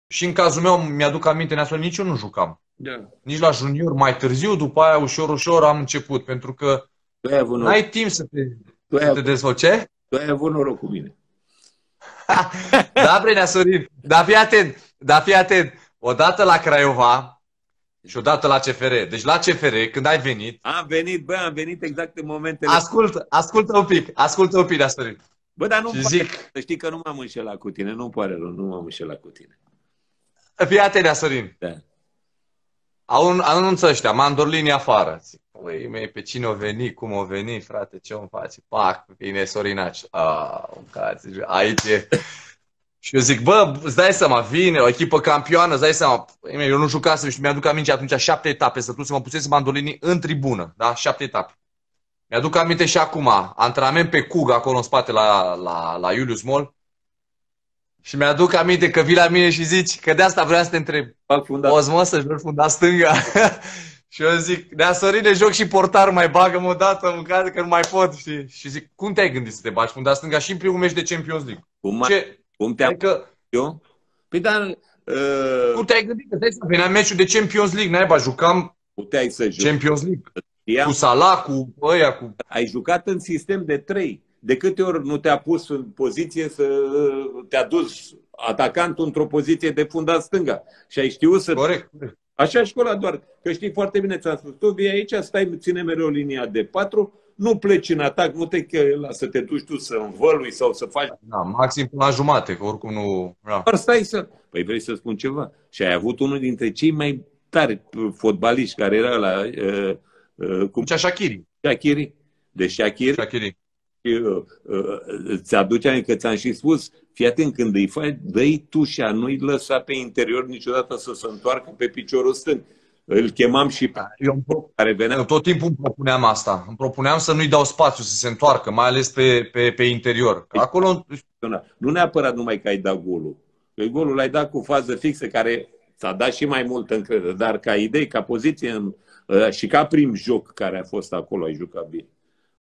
AMINTIRILE LUI IONUT RADA SI SORIN CARTU DIN PERIOADA CFR | IMPACTUL AVUT ASUPRA CARIEREI MELE
✔️Clip extras din transmisiunea live de pe Instagram
‼️ Urmariti-ne si pe alte platforme: